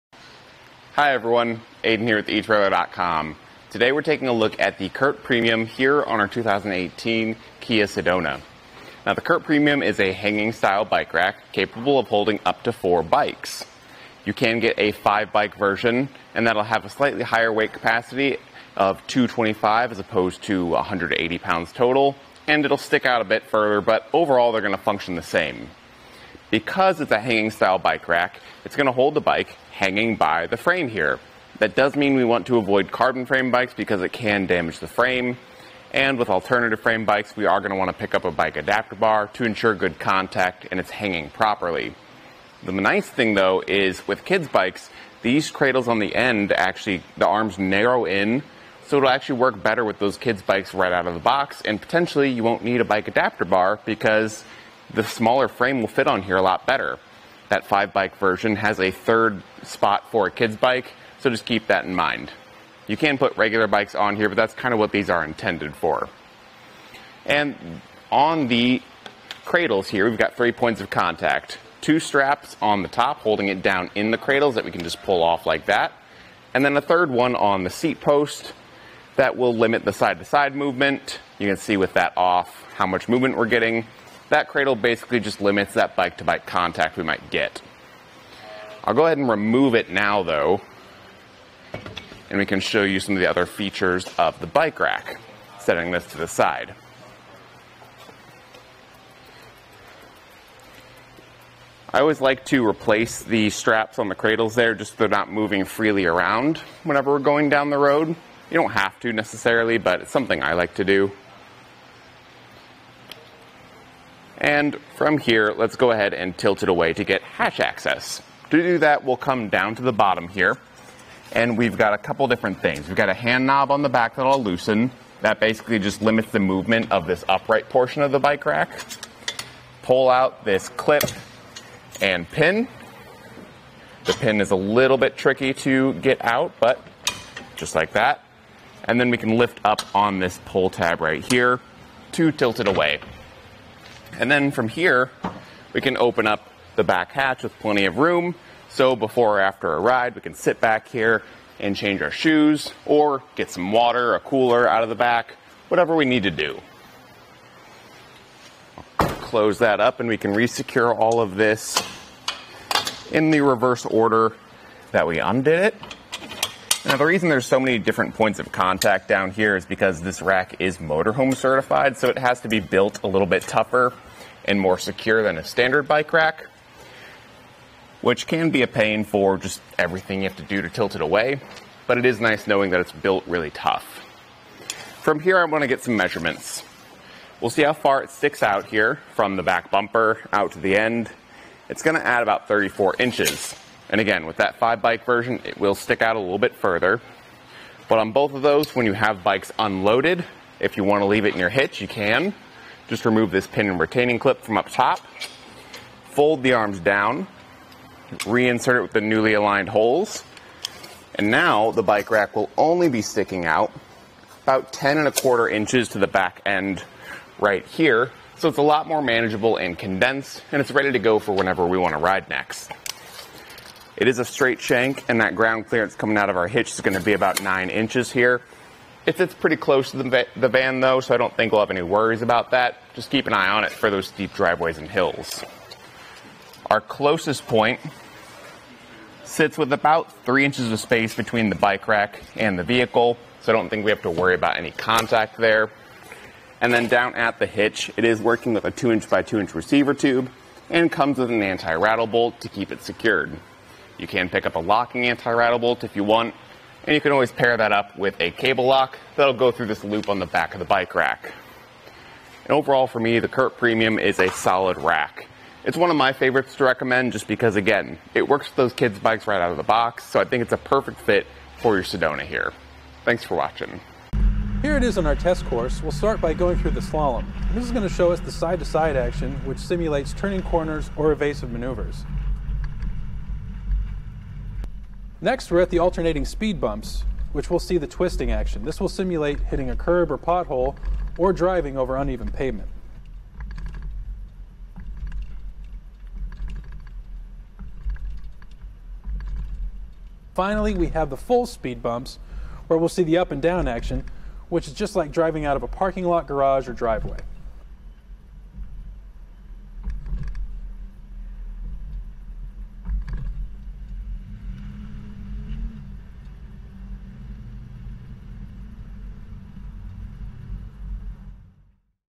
etrailer | Fit Test: Curt Bike Rack for 4 Bikes with 2018 Kia Sedona
Hi everyone, Aidan here at etrailer.com Today, we're taking a look at the Curt Premium here on our 2018 Kia Sedona. Now the Curt Premium is a hanging style bike rack capable of holding up to four bikes. You can get a five bike version, and that'll have a slightly higher weight capacity of 225 as opposed to 180 pounds total. And it'll stick out a bit further, but overall they're gonna function the same. Because it's a hanging style bike rack, it's gonna hold the bike hanging by the frame here. That does mean we want to avoid carbon frame bikes because it can damage the frame.

And with alternative frame bikes, we are gonna want to pick up a bike adapter bar to ensure good contact and it's hanging properly. The nice thing though is with kids bikes, these cradles on the end, actually the arms narrow in. So it'll actually work better with those kids bikes right out of the box. And potentially you won't need a bike adapter bar, because the smaller frame will fit on here a lot better. That five bike version has a third spot for a kids bike, so just keep that in mind.

You can put regular bikes on here but that's kind of what these are intended for. And on the cradles here, we've got three points of contact. Two straps on the top, holding it down in the cradles that we can just pull off like that. And then the third one on the seat post that will limit the side to side movement. You can see with that off, how much movement we're getting.

That cradle basically just limits that bike to bike contact we might get. I'll go ahead and remove it now though. And we can show you some of the other features of the bike rack, setting this to the side. I always like to replace the straps on the cradles there, just so they're not moving freely around whenever we're going down the road. You don't have to necessarily, but it's something I like to do.

And from here, let's go ahead and tilt it away to get hatch access. To do that, we'll come down to the bottom here. And we've got a couple different things. We got a hand knob on the back that I'll loosen, that basically just limits the movement of this upright portion of the bike rack. Pull out this clip and pin. The pin is a little bit tricky to get out, but just like that. And then we can lift up on this pole tab right here to tilt away. And then from here, we can open up the back hatch with plenty of room. So before or after a ride, we can sit back here and change our shoes or get some water, a cooler out of the back, whatever we need to do. Close that up, and we can resecure all of this in the reverse order that we undid it. Now, the reason there's so many different points of contact down here is because this rack is motorhome certified. So it has to be built a little bit tougher and more secure than a standard bike rack. Which can be a pain for just everything you have to do to tilt it away. But it is nice knowing that it's built really tough. From here, I wanna get some measurements. We'll see how far it sticks out here from the back bumper out to the end, it's gonna add about 34 inches. And again, with that five bike version, it will stick out a little bit further. But on both of those, when you have bikes unloaded, if you wanna leave it in your hitch, you can. Just remove this pin and retaining clip from up top, fold the arms down, reenter it with the newly aligned holes. And now the bike rack will only be sticking out about 10 1/4 inches to the back end right here. So it's a lot more manageable and condensed, and it's ready to go for whenever we wanna ride next. It is a straight shank and that ground clearance coming out of our hitch it's gonna be about nine inches here. It sits pretty close to the van though, so I don't think we'll have any worries about that. Just keep an eye on it for those steep driveways and hills. Our closest point sits with about three inches of space between the bike rack and the vehicle. So I don't think we have to worry about any contact there. And then down at the hitch, it is working with a two inch by two inch receiver tube and comes with an anti-rattle bolt to keep it secured. You can pick up a locking anti-rattle bolt if you want. And you can always pair that up with a cable lock so it'll go through this loop on the back of the bike rack. And overall for me, the Curt Premium is a solid rack. It's one of my favorites to recommend just because again, it works for those kids bikes right out